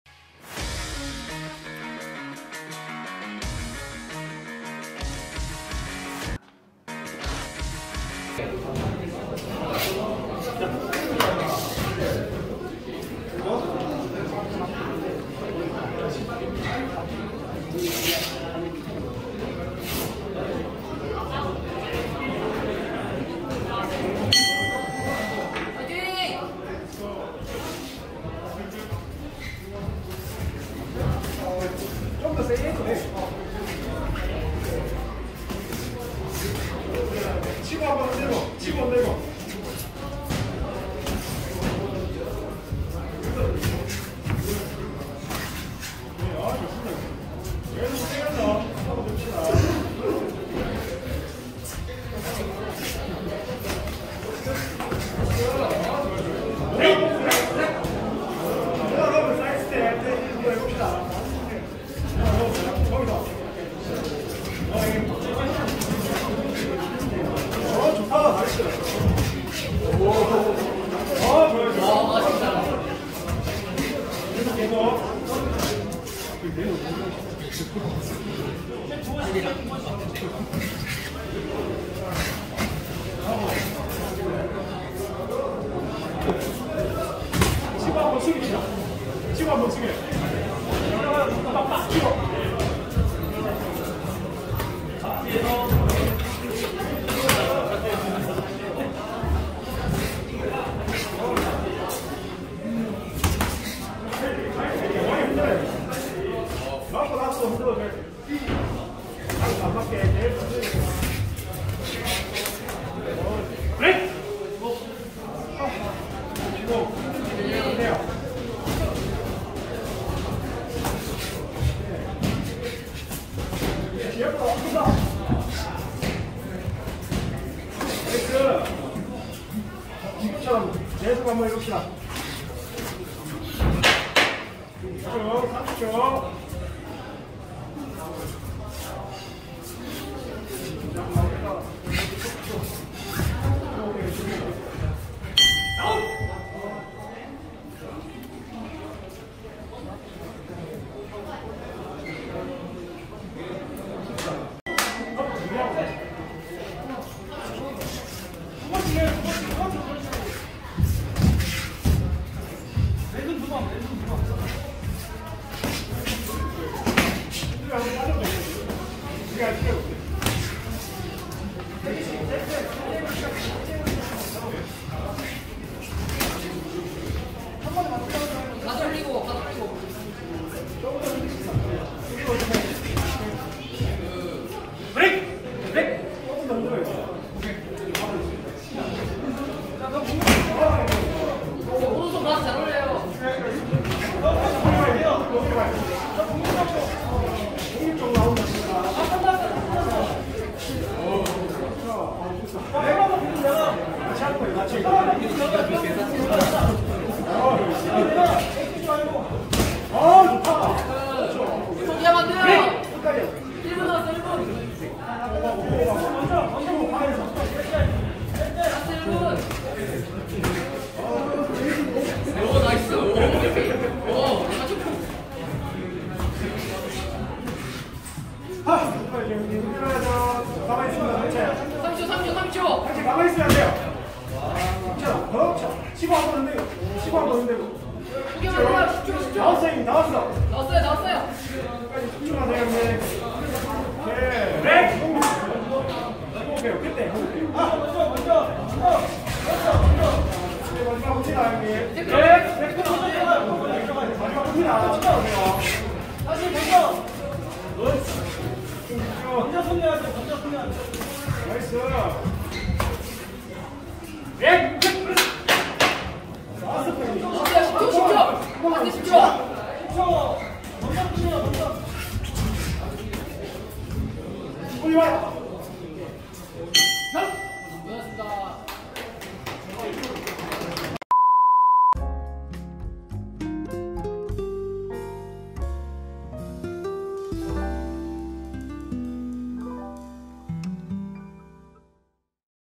복싱 시합은 스파링과 다르네 ~ 장정구 서열 5위 프로 테스트
양정 장정구 서열 5위 프로 테스트에 자신있게 나서다  "시합은  스파링과 다르네예 ~